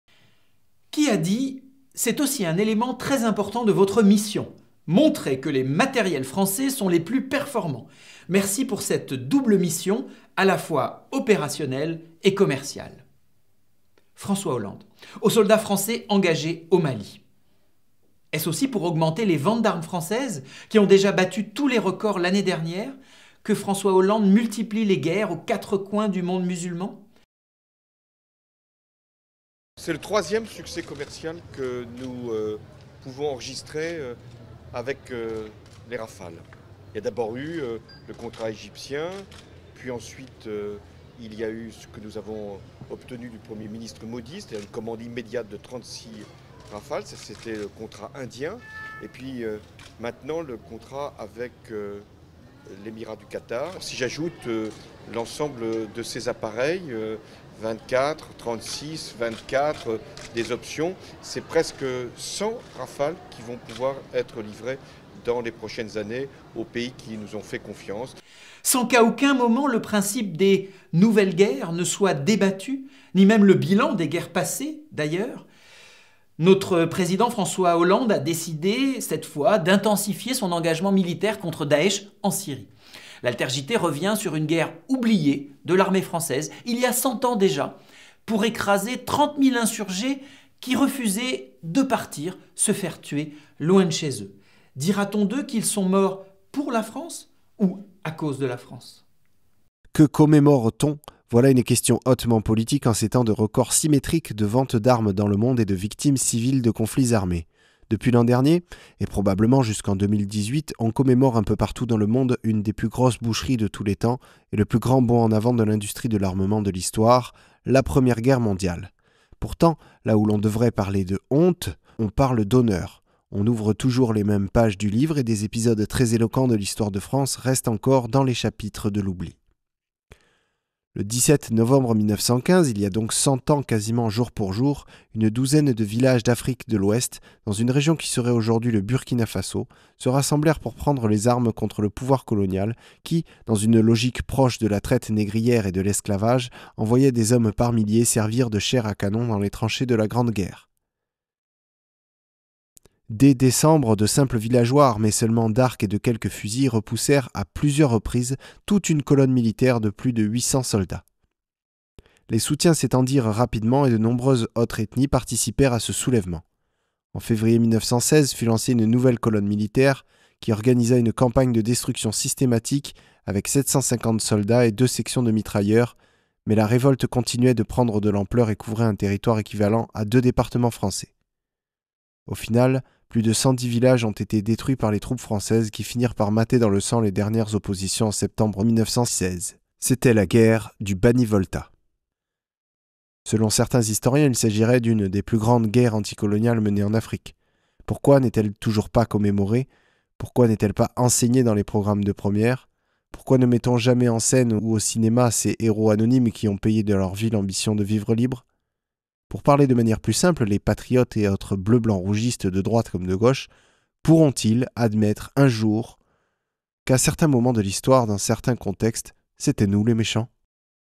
"Mort par la France" : la Guerre du Bani-Volta - AlterJT #87
30 000 individus massacrés par la France. Il y a deja 100 ans, alors que la Première Guerre Mondiale faisait rage, l'armée française s'est rendue coupable d'un pur massacre. Des dizaines de milliers d'hommes qui refusaient simplement de partir se faire tuer loin de chez eux.
Dira-t-on d'eux qu'ils sont mort "pour" la France ou "à cause" de la France ?